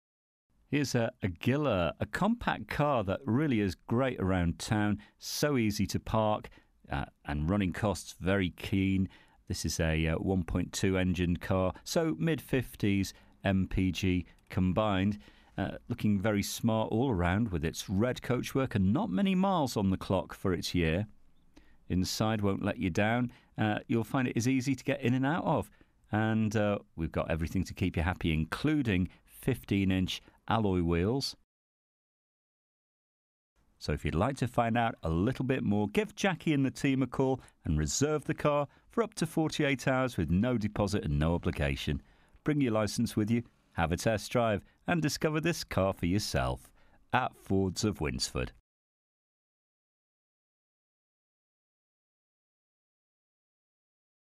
VAUXHALL SE (2013) 1.2 Vvt Ecoflex Se 5dr
DETAILS:
First registered: 01/02/2013
Current mileage: 19800
Registration: YY62JKF
Colour: Red

This used car is for sale at Fords of Winsford Car Supermarket in Cheshire, UK